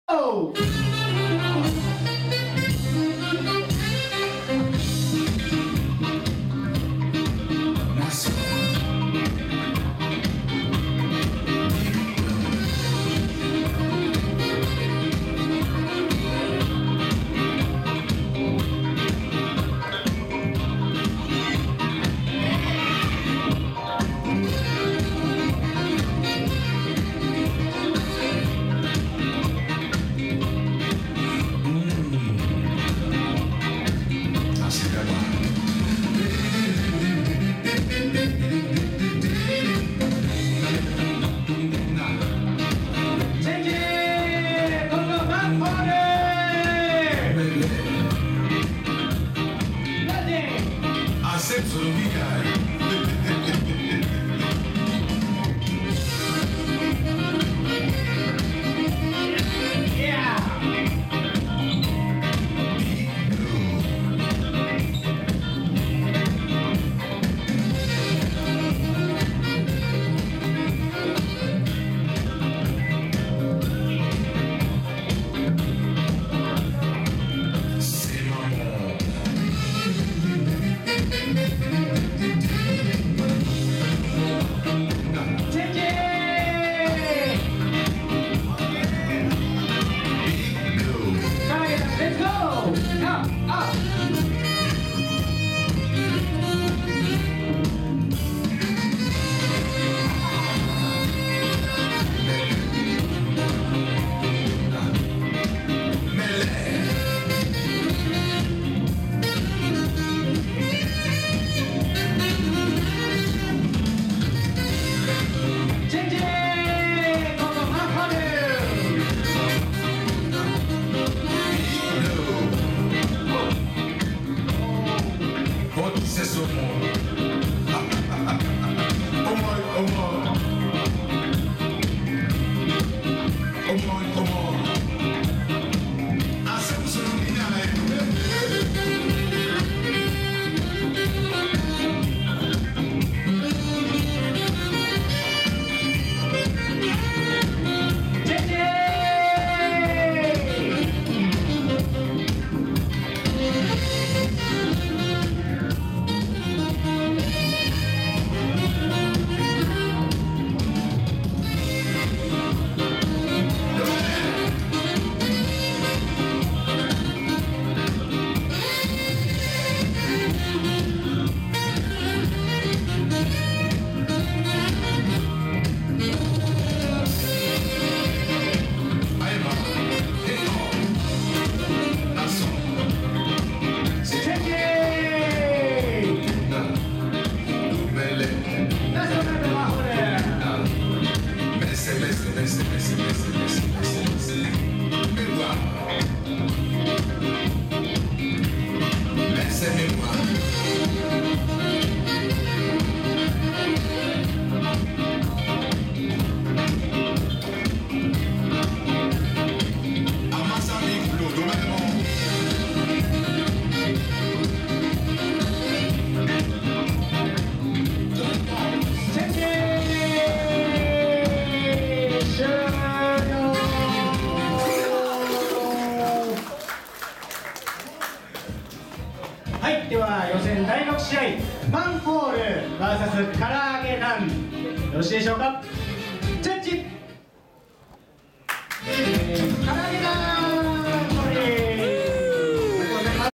OSY5 33 敗者復活BATTLE MAN HOLE vs からあげ団
OLD SCHOOL YUUGATA vol.5
2010/05/16(SUN)
Pineapple Studio Chihaya, Nagoya, Japan